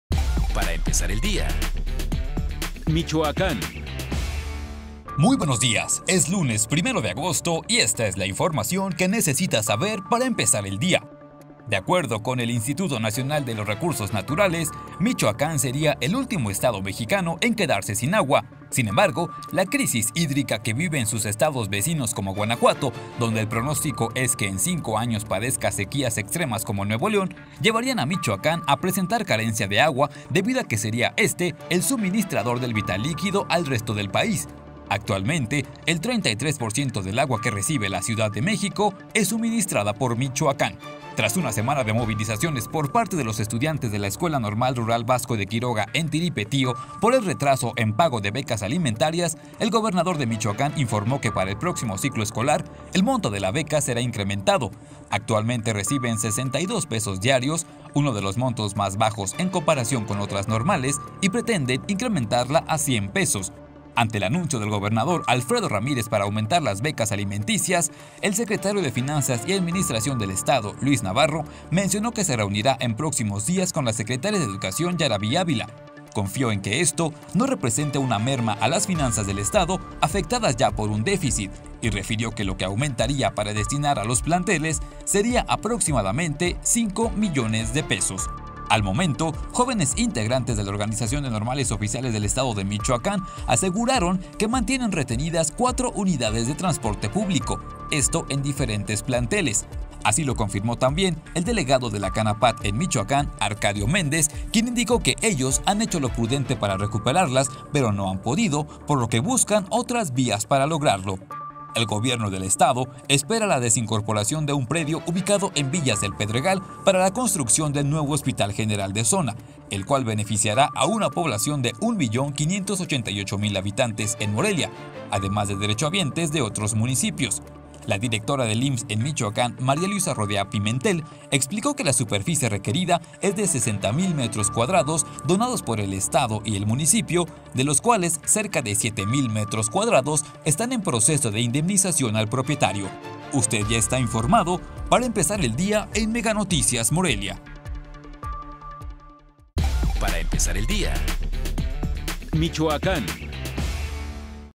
PARA EMPEZAR EL DIA 1 DE AGOSTO
¡Muy buenos días! es lunes 1 de agosto y esta es la información que necesita saber para empezar el día:

De acuerdo con el Instituto Nacional de los Recursos Naturales, Michoacán sería el último estado mexicano en quedarse sin agua, sin embargo, la crisis hídrica que viven sus estados vecinos como Guanajuato donde el pronóstico es que en cinco años padezca sequías extremas como Nuevo León, llevarían a Michoacán a presentar carencia de agua, debido a que sería este el suministrador del vital líquido al resto del país. El 33% del agua que recibe la Ciudad de México es suministrada por Michoacán.

Tras una semana de movilizaciones por parte de los estudiantes de la Escuela Normal Rural Vasco de Quiroga en Tiripetío, por el retraso en pago de becas alimentarias, el gobernador de Michoacán informó que para el próximo ciclo escolar el monto de la beca será incrementado. Actualmente reciben 62 pesos diarios, uno de los montos más bajos en comparación con otras normales, y pretenden subirla a 100 pesos.

Ante el anuncio del gobernador Alfredo Ramírez para aumentar las becas alimenticias que se da a los estudiantes normalistas, las autoridades involucradas analizan la viabilidad de ello. El secretario de Finanzas y Administración de Michoacán, Luis Navarro, mencionó que se reunirá en próximos días con la secretaria de Educación, Yarabí Ávila; confió en que esto no represente una merma a las finanzas del estado, afectadas por un déficit, y refirió que lo que aumentaría para destinar a los planteles sería aproximadamente 5 millones de pesos.

Al momento, los jóvenes integrantes de la Organización de Normales Oficiales del Estado de Michoacán (ONOEM) mantienen aún retenidas cuatro unidades de transporte público, en diferentes planteles. El delegado de la Canapat en Michoacán, Arcadio Méndez, indicó que ellos han hecho lo prudente para recuperarlas, pero no han podido, por lo que buscan otras vías para lograrlo.

El Gobierno del Estado espera la desincorporación de un predio ubicado en Villas del Pedregal, para la construcción del nuevo Hospital General de Zona, el cual beneficiará a una población de 1 millón 588 mil habitantes en Morelia, además de derechohabientes de otros municipios. La directora del IMSS, María Luisa Rodea Pimente, explicó que la superficie requerida es de 60,000 metros cuadrados, donados por estado y municipio, de los cuales cerca de 7 mil metros cuadrados están en proceso de indemnización al propietario

Usted ya está informado, para empezar el día en MeganoticiasMorelia